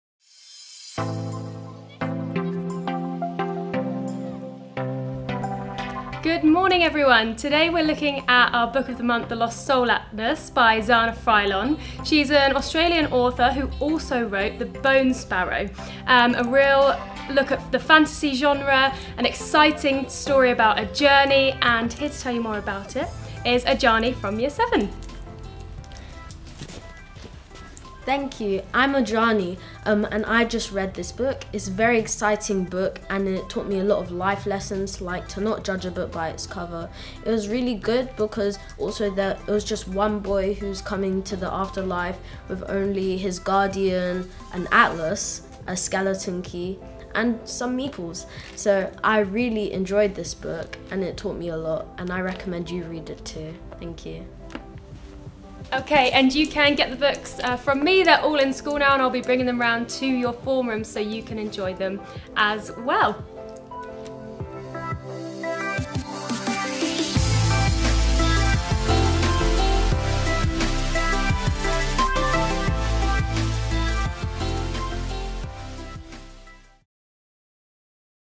Book of the Month - The Lost Soul - Zana Fraillon
A boy awakens in the Afterlife, with a pocketful of vague memories, a key, a raven, and a mysterious atlas to guide him as he sets out to piece together what happened, and try to find his way home.